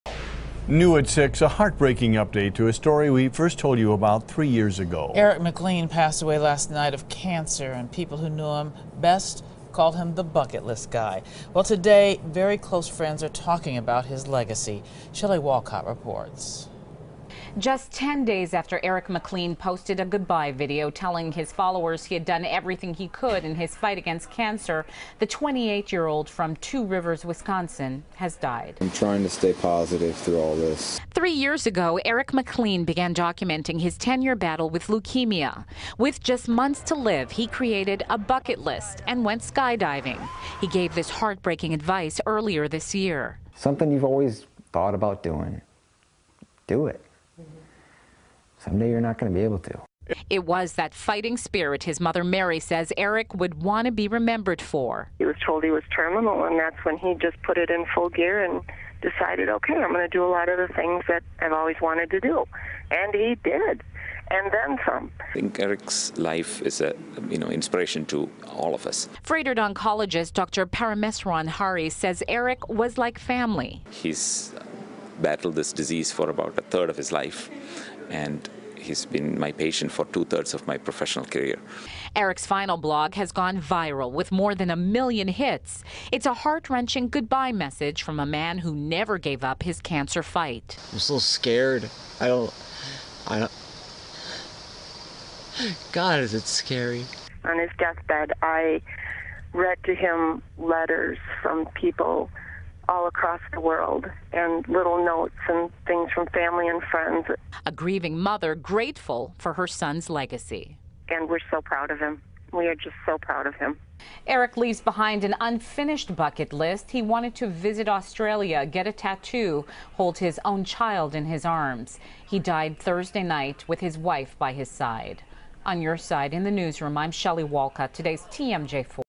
Bucket List Guy Eric McLean Passes Away
Shelley Walcott reports